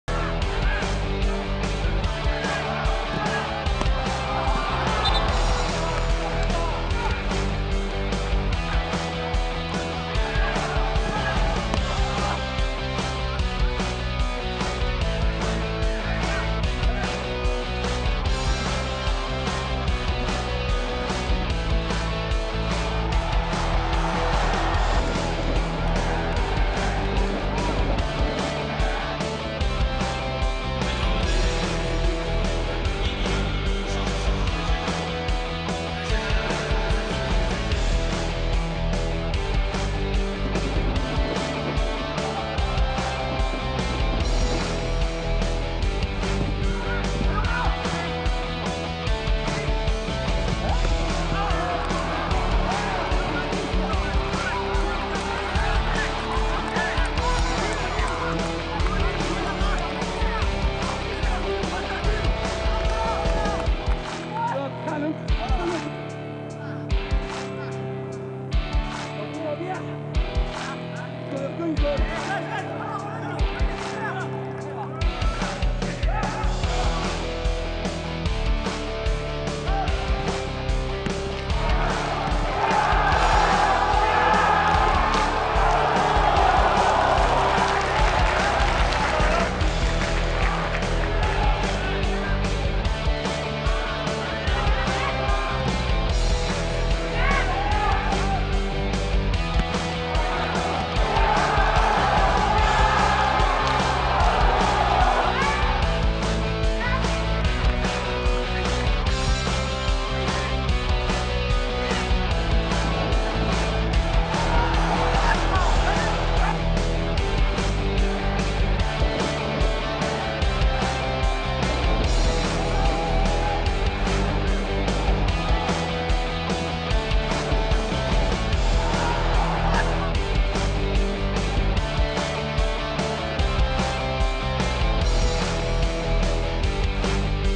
Chinese Super League: Top 5 Saves of 11th round |2020 中超联赛 第十一轮五佳扑救
Zhang Chong (Dalian Pro)
Yan Junling (Shanghai SIPG)
Liu Zhenli (Qingdao Huanghai)
Zeng Cheng (Shanghai Shenhua)